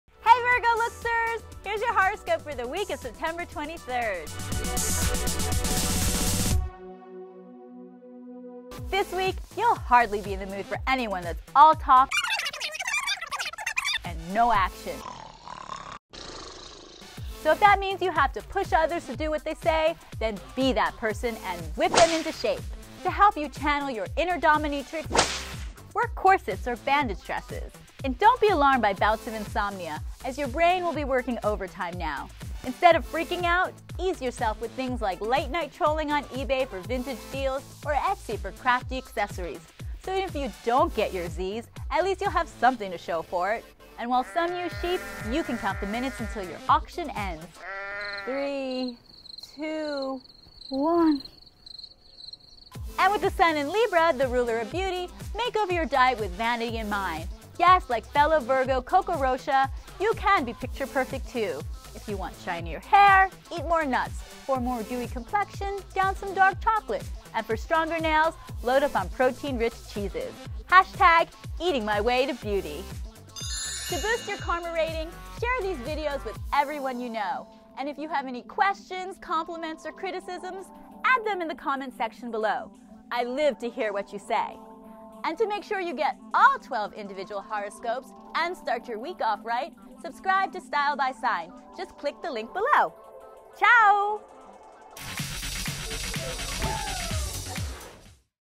Virgo Horoscope: Style By Sign Week of September 23rd
Style advice based on your personal horoscope. What do the stars have in store for your sign this week? Find out here on Look TV's Style By Sign!